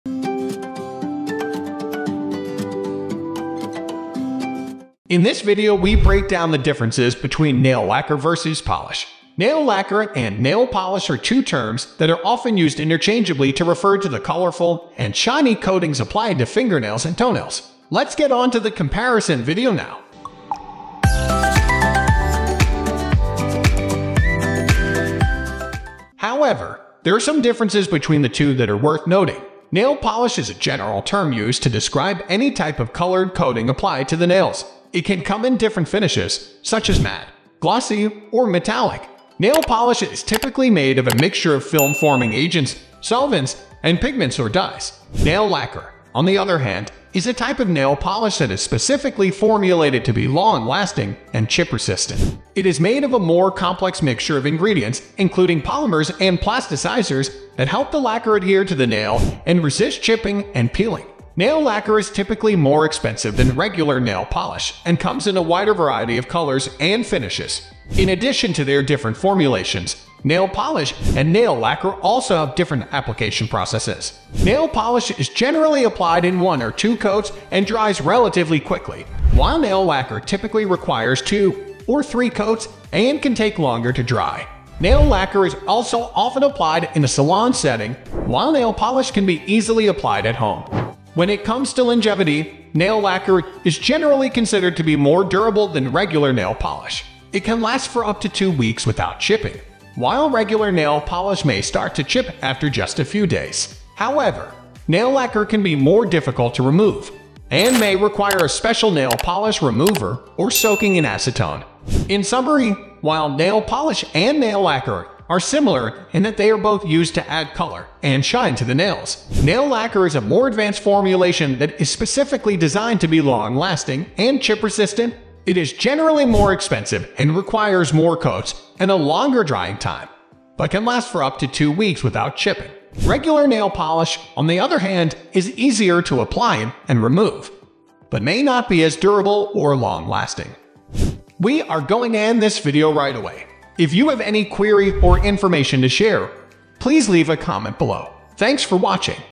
Nail Lacquer vs Nail Polish: What's the Difference? [2024]
Welcome to our YouTube video where we explore the key differences between nail lacquer and nail polish! If you've ever wondered which product is right for you or why they're labeled differently, this video has got you covered. Our expert guide will break down the characteristics, finishes, and application techniques of both nail lacquer and nail polish, helping you make an informed decision for your next manicure.

In this comprehensive comparison, we'll discuss the longevity, pigmentation, and formula of each product, highlighting their unique advantages and considerations. Whether you're a nail care enthusiast, a professional nail artist, or simply someone who loves experimenting with different looks, this video is for you!